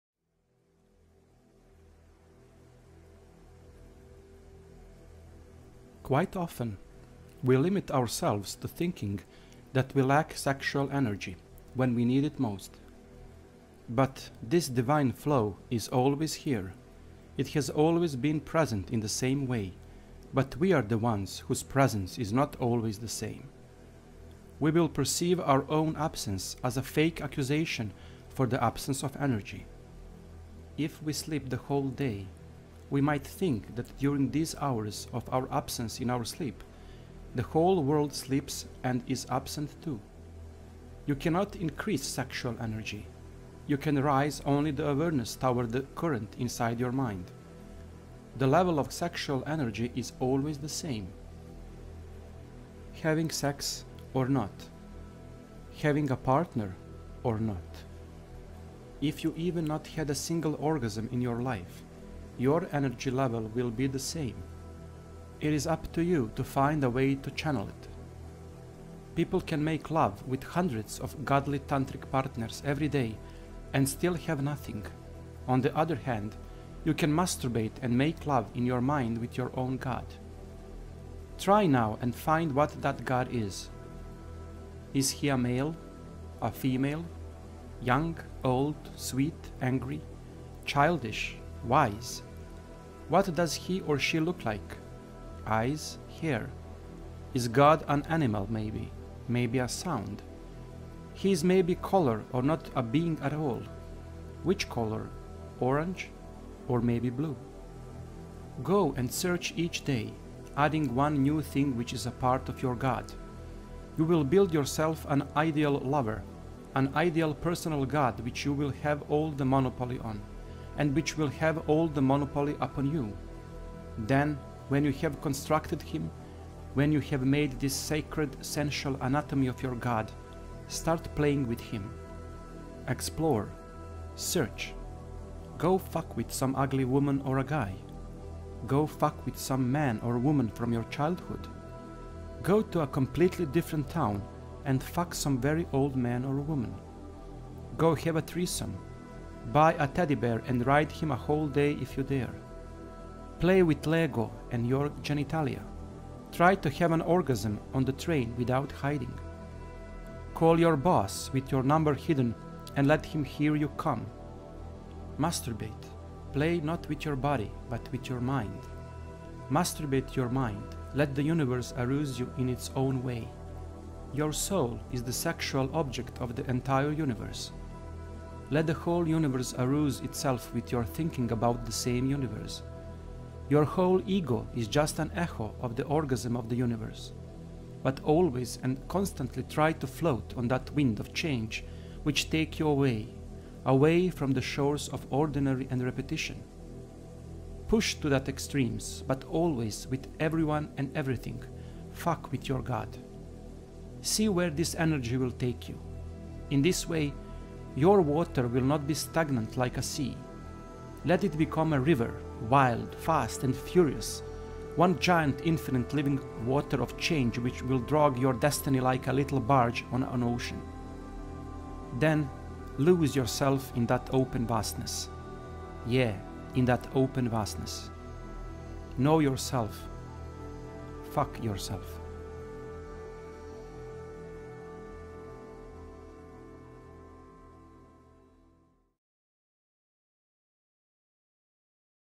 On sexual energy
On sexual energy, by Frater 273. Special acknowledgement to David Chaim Smith and his wonderful art.